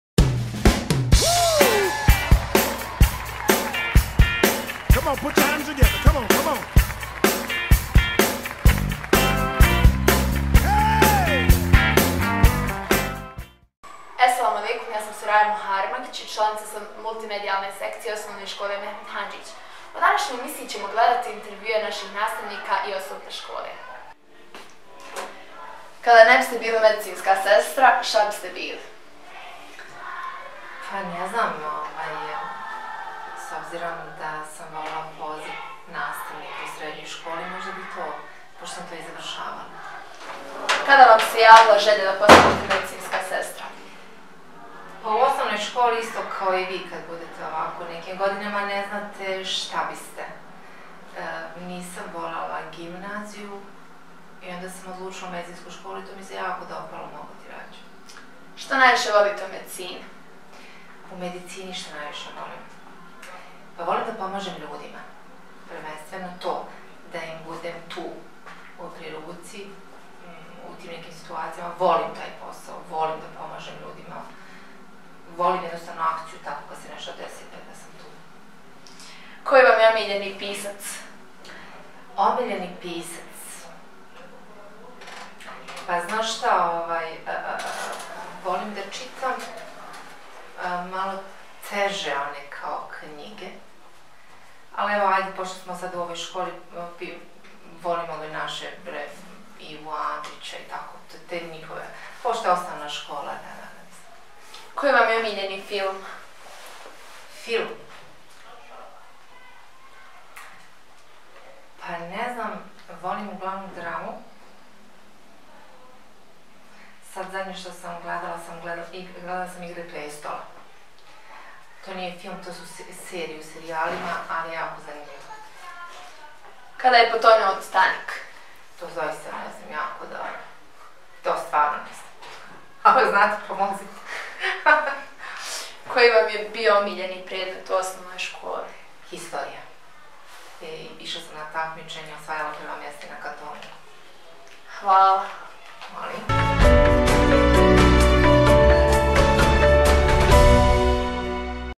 Brza pitalica - medicinska sestra Sabina Škrijelj
U današnjoj emisiji Brze pitalice pogledajte intervju naše učenice Soraje Muharemagić sa medicinskom sestrom Sabinom Škrijelj.